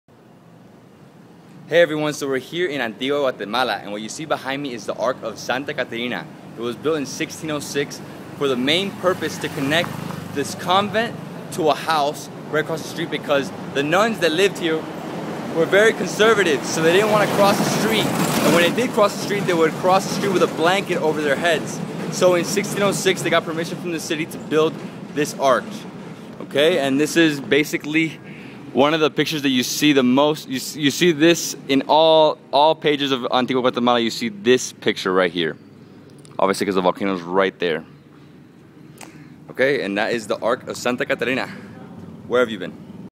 The Arch of Santa Katarina - Antigua, Guatemala
In this iPhone Short, DBH brings us to the central highland town of Antigua, Guatemala, where David displays the Arch of Santa Catalina. Built in 1606, this arch was initially built to connect a convent to a neighboring house, so the conservative nuns wouldn't have to cross the street in public. With the Volcano de Agua directly in the background, this arch has become one of Antigua's biggest icons. Visit this beautiful 17th Century arch and its stunning volcanic background with the DBH Guide to Guatemala, now available for your Kindle as well.

SEND ME STUFF:
USA